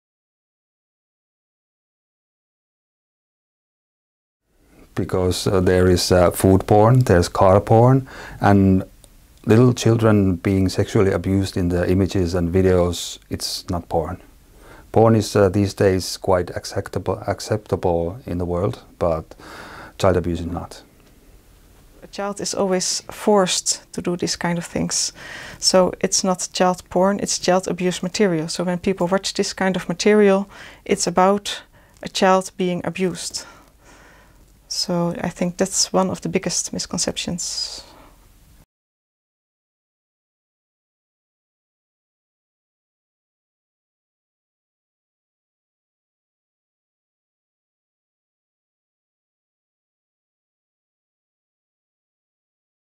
Why is the term "child porn" unacceptable?
At INTERPOL, we support the use of appropriate terminology to avoid trivializing the sexual abuse and exploitation of children. In this video, members of INTERPOL's Crimes Against Children unit explain why the term "child porn" should not be legitimate language used by law enforcement, judiciary, the public or media.